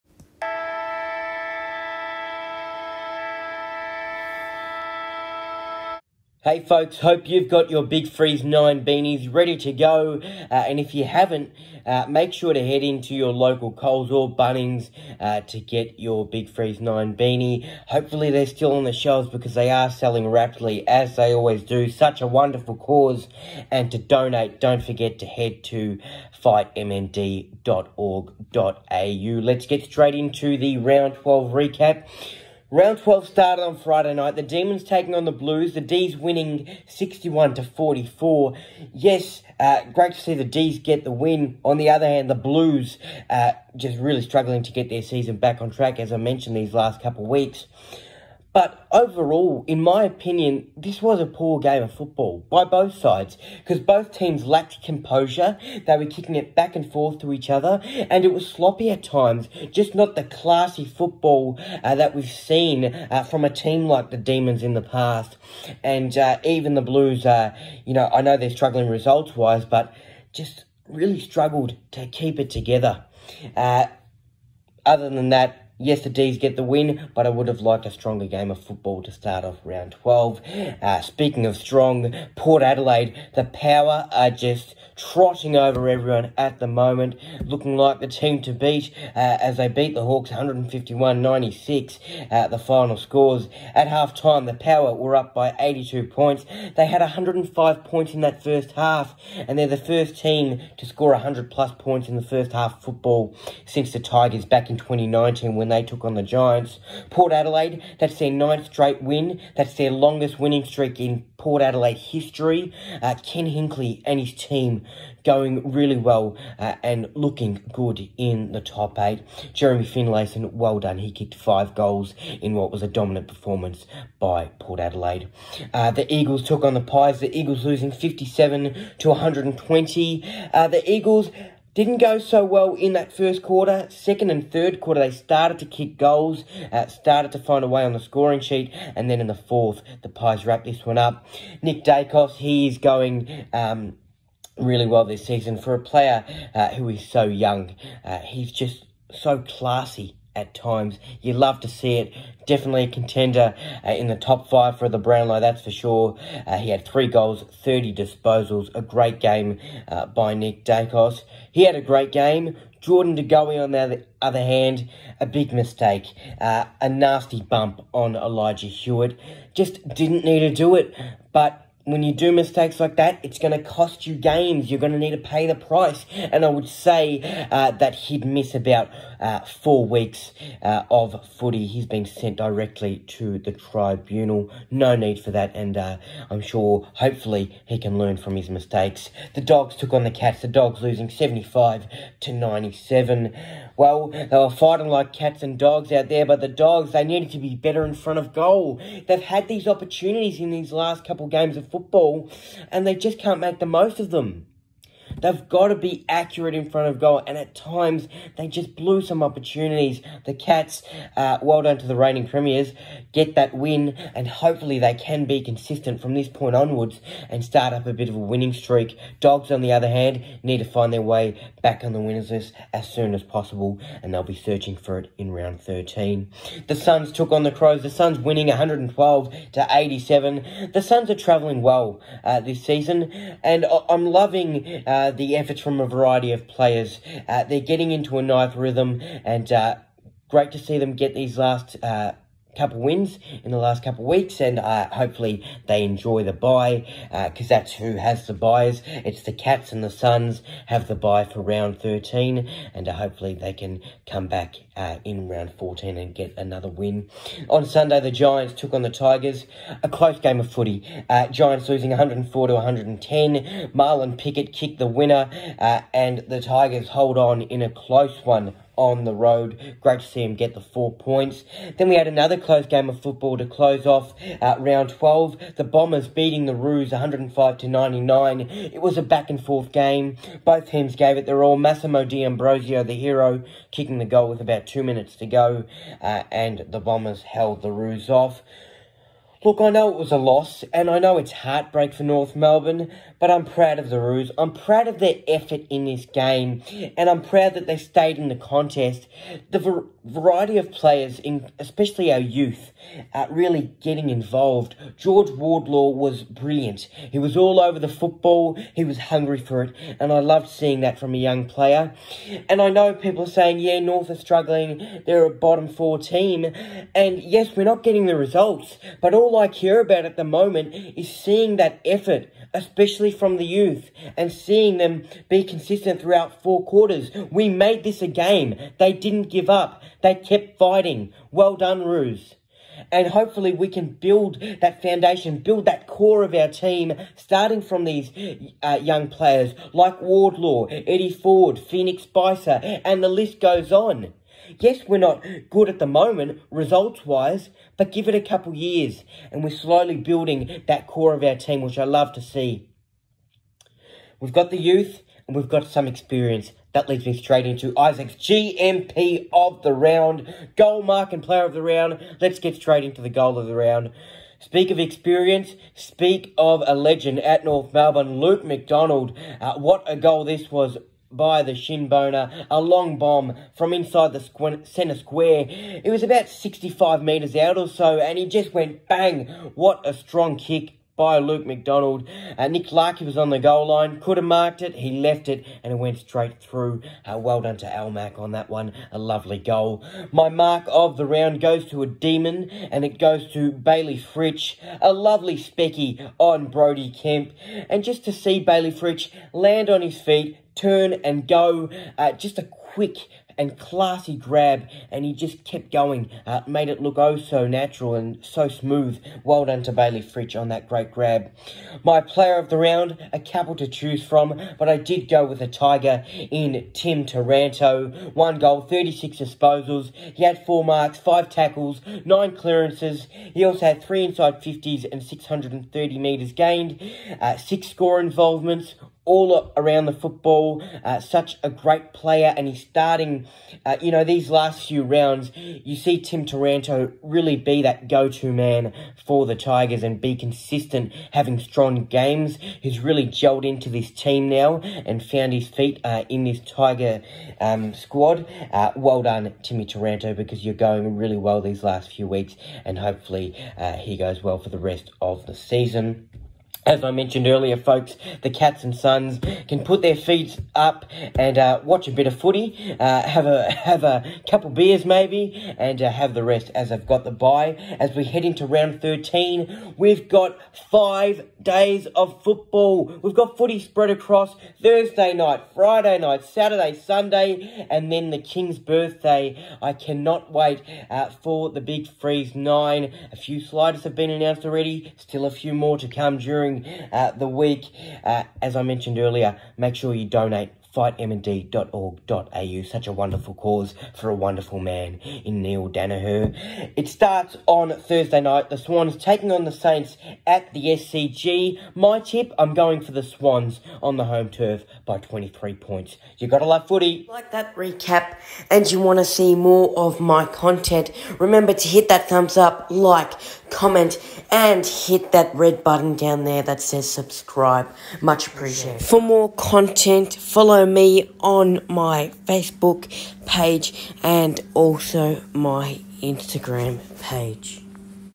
AFL Round 12 Recap
Hey Footy fans here is the Round 12 Recap. I can’t wait for Round 13 because we’ve got footy across five days and we finish the round with the wonderful Big Freeze 9. Round 13 starts on Thursday night when the Swans take on the Saints at the SCG.
Gotta Love Footy!